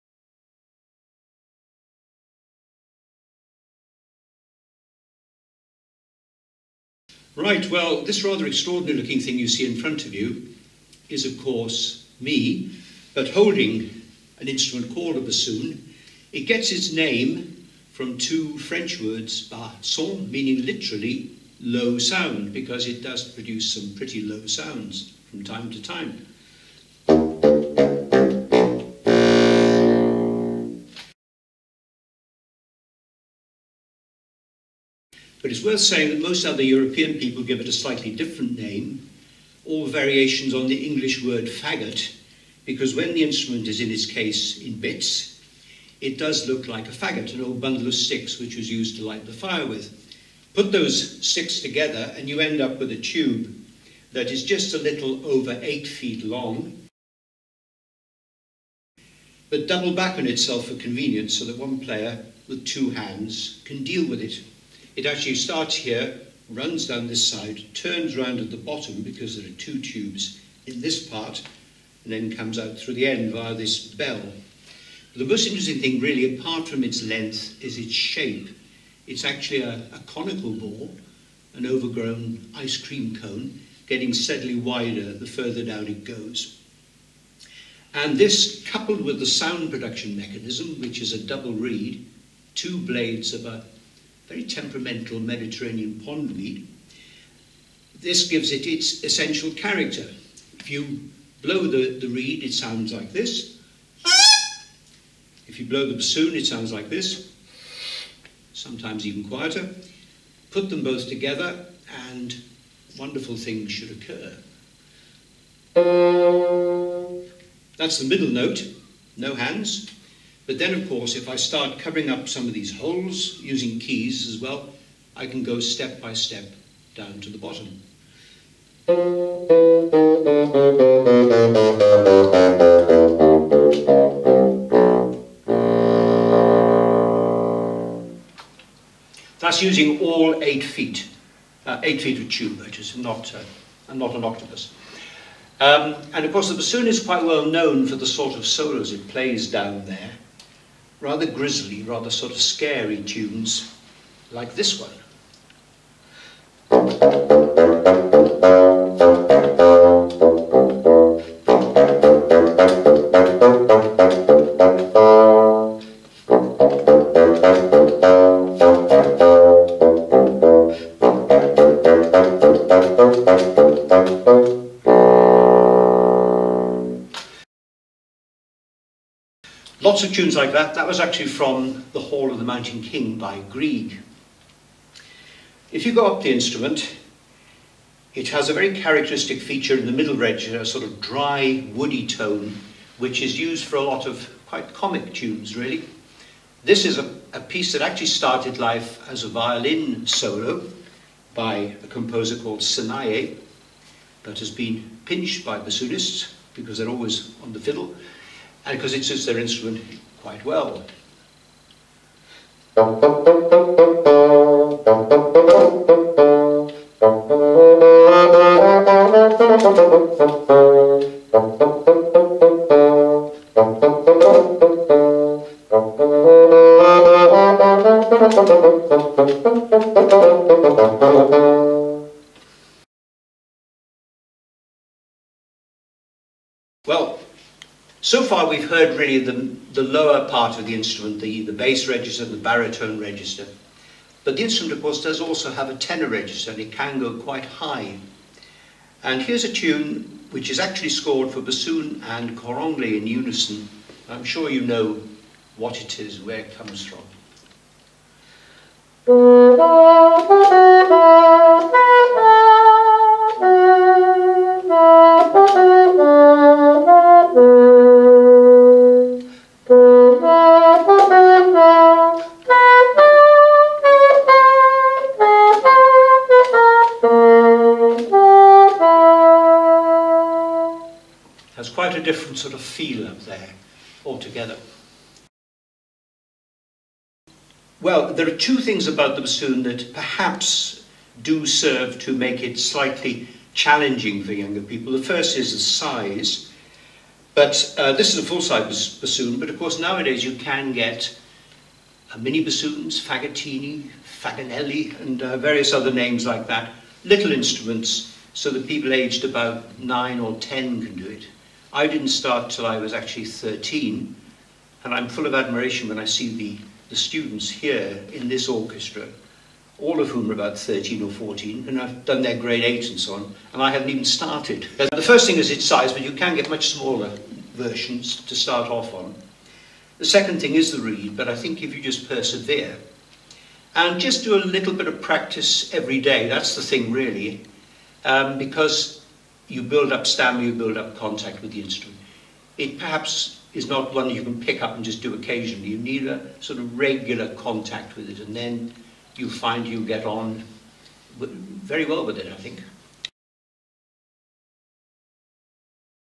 A beginner's guide to the bassoon
Do you fancy learning an instrument that is 8 foot long (if it was straightened!)? We were amazed too! We asked Bob Codd, one of NCO's Bassoon tutors, to introduce us to this fascinating woodwind instrument.